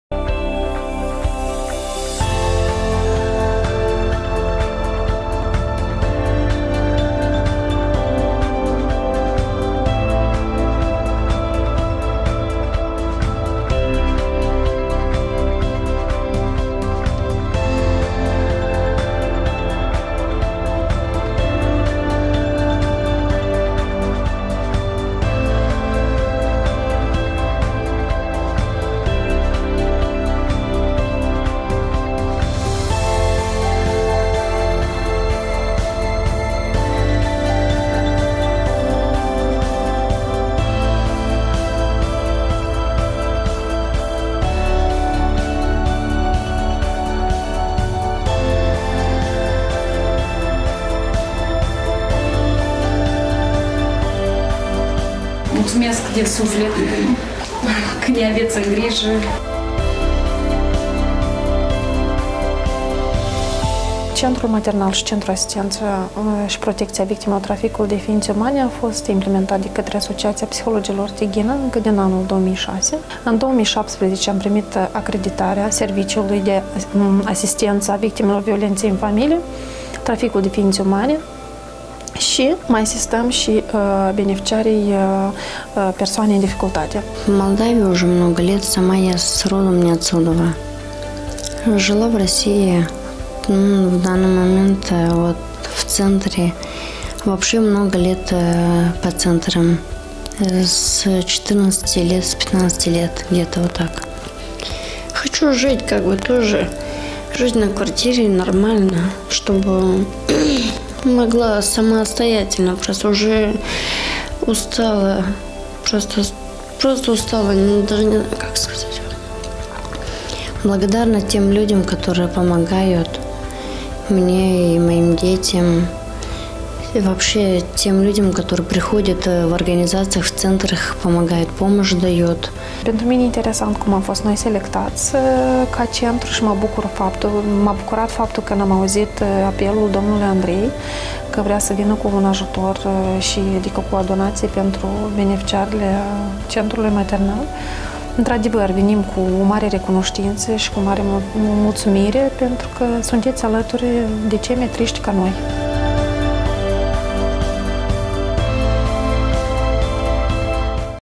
CREDEM CĂ ESTE IMPORTANT SĂ NE IMPLICĂM ACOLO UNDE ESTE NEVOIE DE NOI!
Vă îndemnăm să ieșim din zona noastră de confort și să privim în jur. Alături de noi, la limita existenței umane, își duc viața oameni care au nevoie de ajutorul și grija noastră. Oameni reali, cu istorii neregizate, pline de dramatism, cu nevoile, durerile, dar și cu micile lor bucurii, atât de rare, de altfel...

V-am obișnuit deja cu campaniile sociale în care ne implicăm pentru a sprijini și a ajuta acolo, unde este nevoie. Cu susținerea partenerilor noștri Similac și BabyLu, sperăm să aducem o rază de speranță și în viața eroilor noștri, oameni cu demnitate, dar care se confruntă cu lipsuri, violență, cu dureri. Probabil, nu le vom putea schimba în totalitate viața, dar, implicându-ne, vom reuși, cel puțin, să le dăm un strop de fericire.

SPUNE STOP VIOLENȚEI! DĂRUIEȘTE IUBIRE!